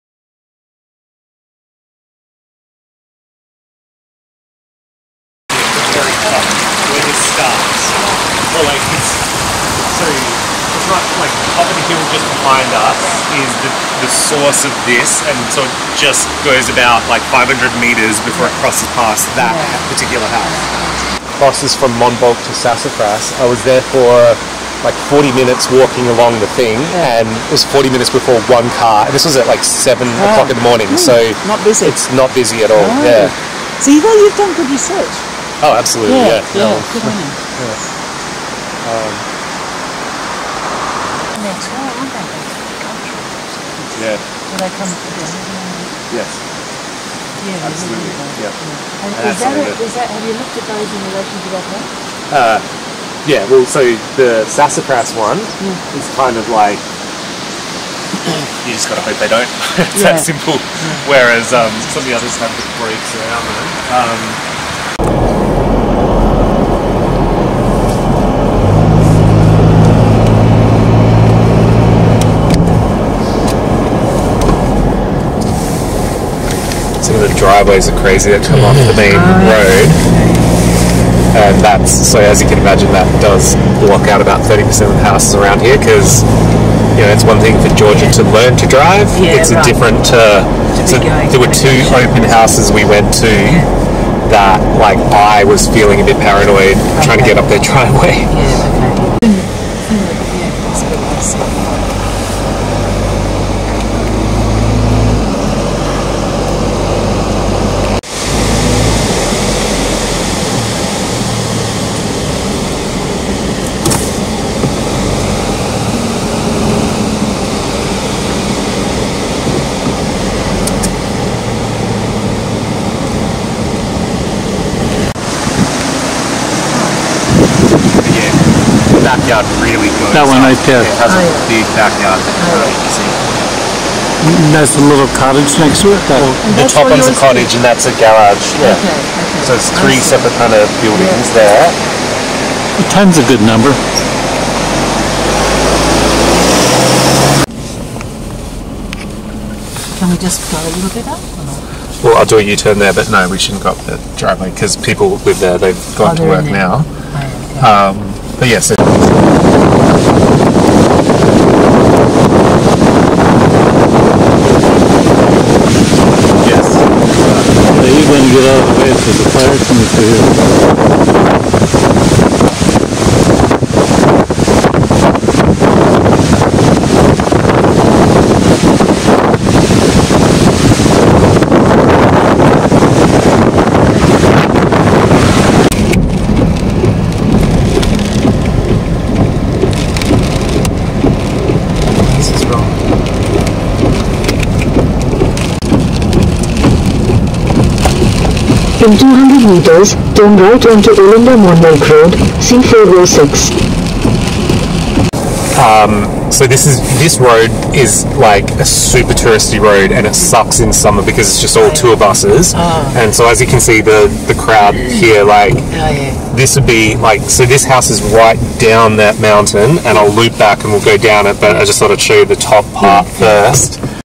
New home in the Sassafras Creek Nature Reserve between Belgrave and Monbulk Victoria Australia
New Home near Belgrave and , part of the Shire of Yarra Ranges the Dandenong Ranges.
Home of Puffing Billy Railway - The Puffing Billy Railway is a 2 ft 6 in narrow gauge heritage railway in the southern foothills of the Dandenong Ranges in Melbourne, Australia.